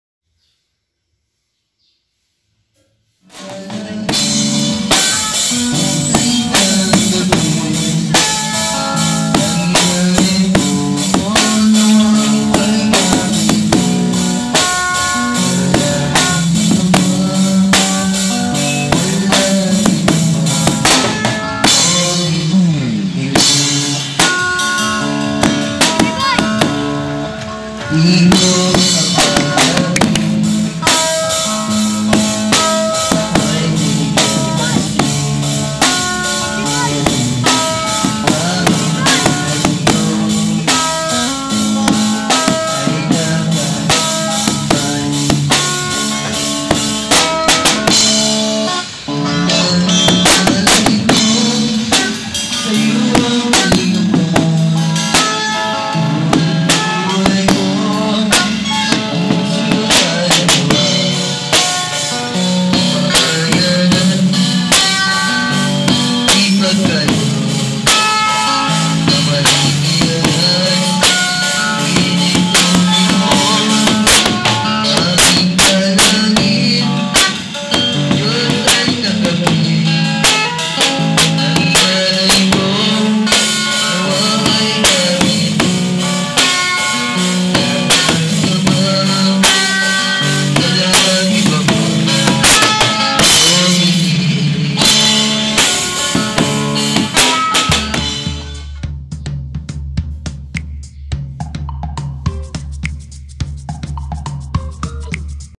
Ang Little Drummer Girl ng Baclaran
Ang batang babae na ito ay aking kinuhanan ng video habang tumutugtog siya ng drums at kumakanta naman sa tabi niya ang isang lalaki. Natagpuan ko sila sa gitna ng kalsada sa may dulo ng LRT1 sa Baclaran. Nakakatuwa na kahit sira sira na ang drums ay nagagamit pa niya ito sa pagperform. Panlilimos pa ba ang tawag dito? siguro hindi na, parang nagpeperform na lang sila at ang mga tao naman ay nagbabayad sa panonood sa kanila. Hindi perpekto ang pagtugtog ng bata, mukhang uwido lamang pero kahanga hanga pa din. Sana nag aaral pa ang batang ito.